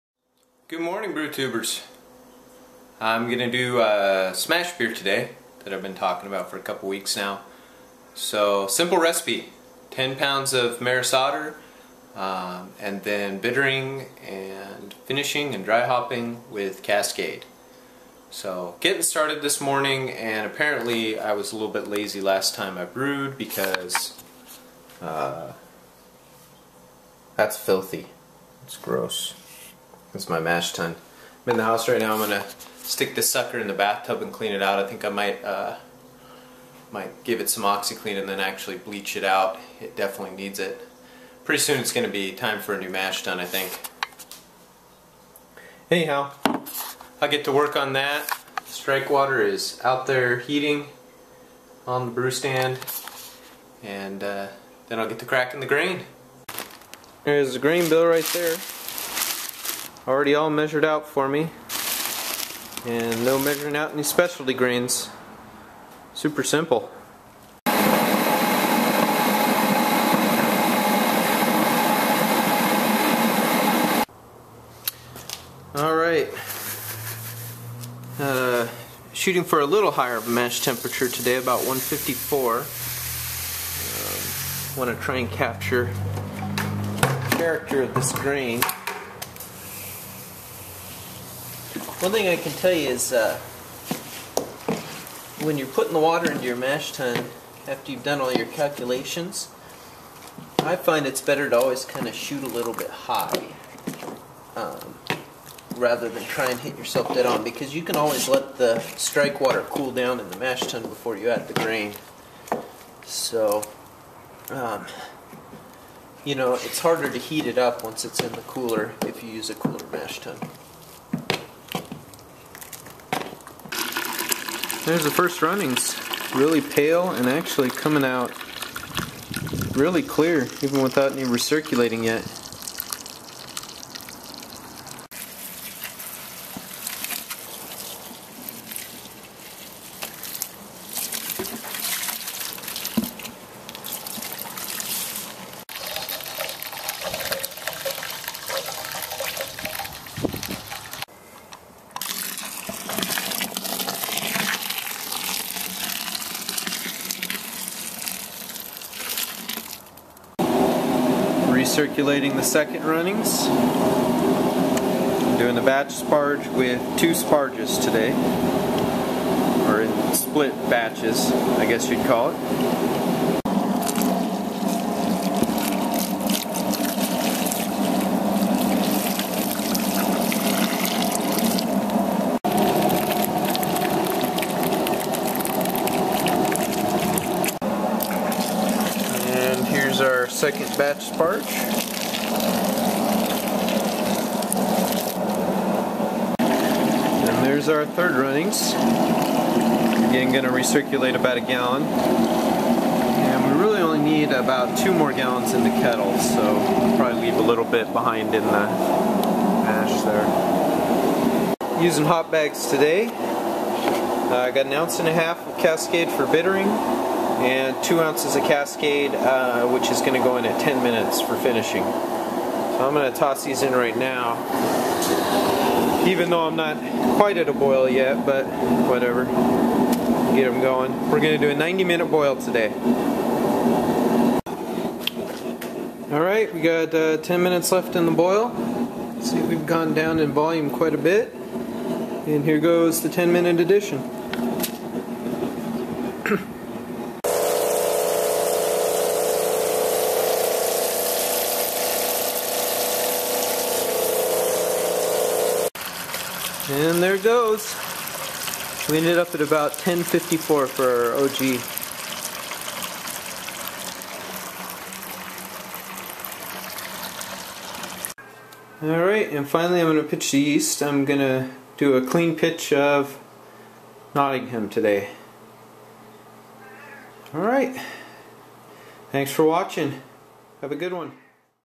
Brewday 2-18-12 (Home Brewing)
Brewed a SMaSH beer today:

5.5 gal 1.054 o.g. 52 IBUs
10# Marris Otter Pale Malt
1.5oz cascade (90 min)
2oz cascade (10 min)
2oz cascade (dry)

Nottingham Ale Yeast